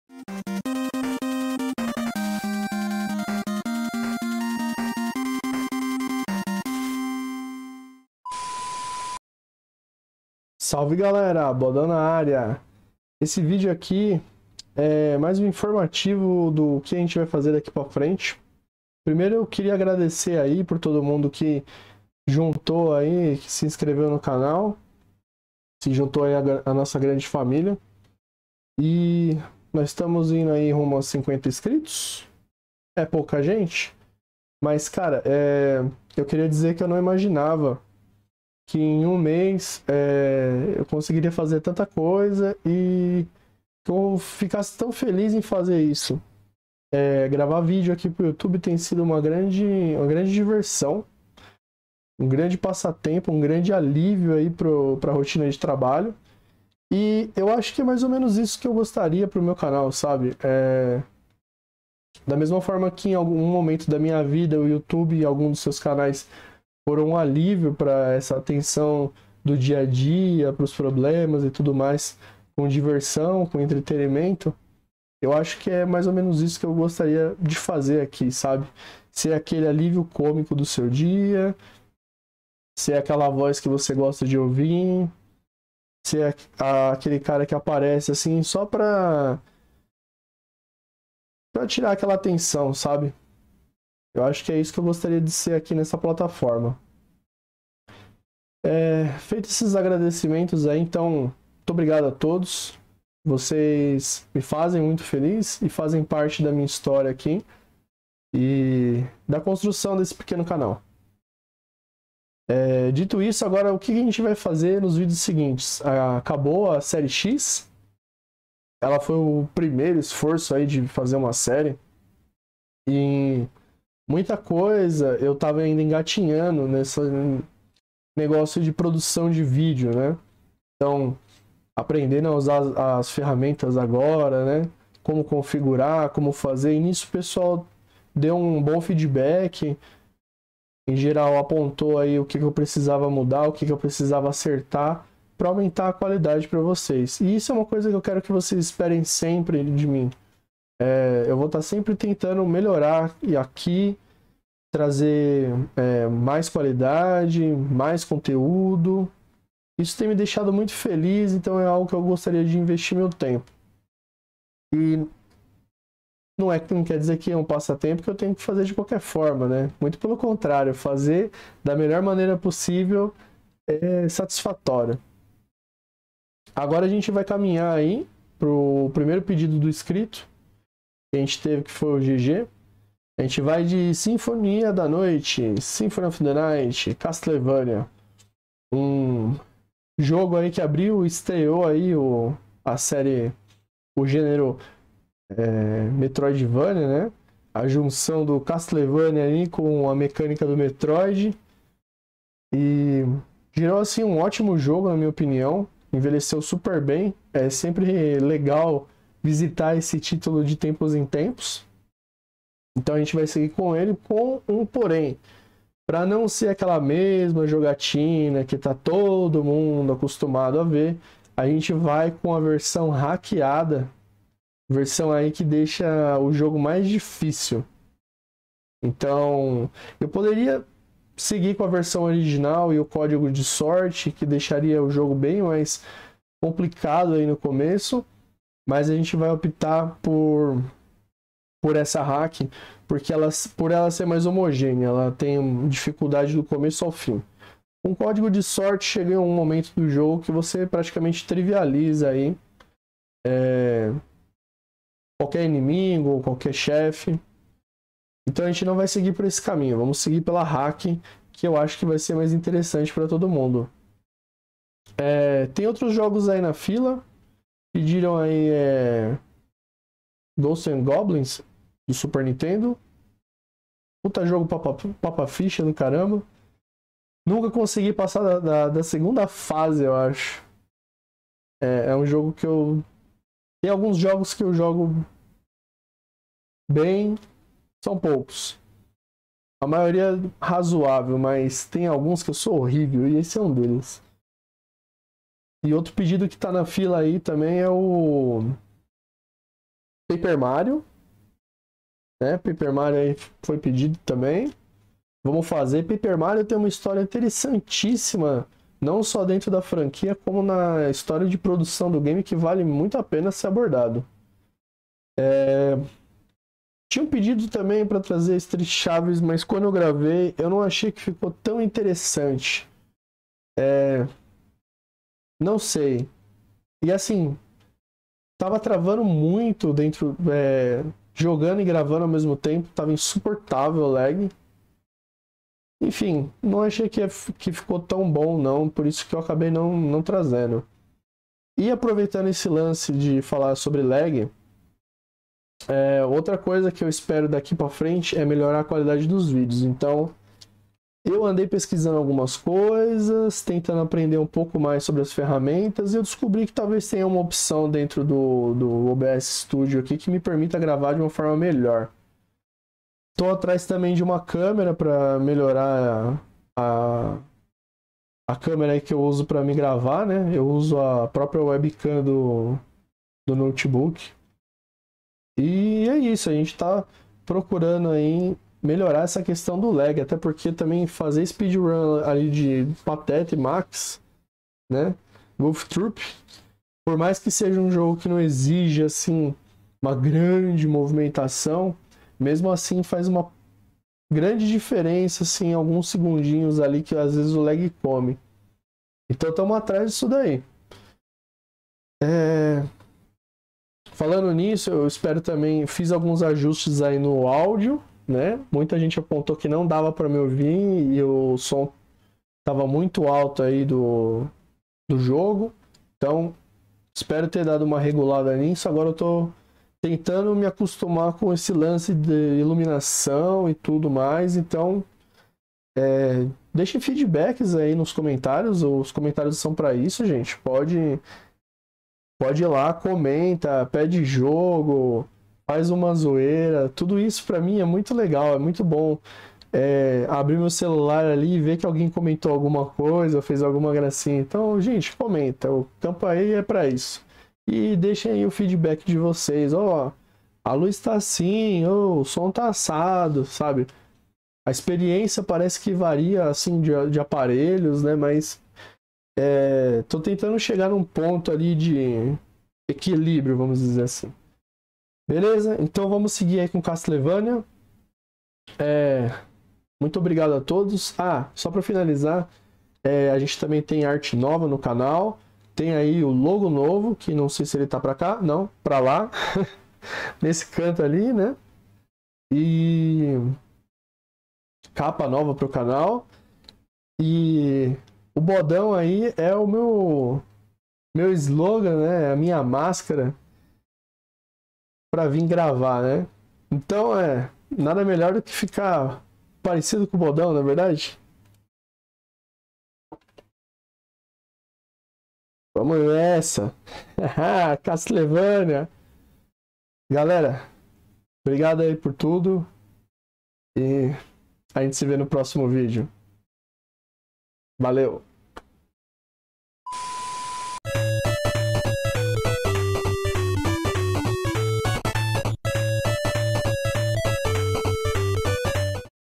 Alguns avisos!!! - Bodão Real APARECEU!!!
Antes de seguirmos com nossas séries, um breve agradecimento, alguns avisos e uma surpresa. Fica até o final pra ver. Só um pequeno adendo, teve um erro de pronuncia onde eu digo "mim", quando na verdade o correto seria "Eu". Tava um pouco nervoso e errei na hora de falar. Obrigado por todos, vocês fazem esse canal ser tão fantástico para mim.

Quer me dar um trocado pra ajudar no canal? Só se não fizer falta!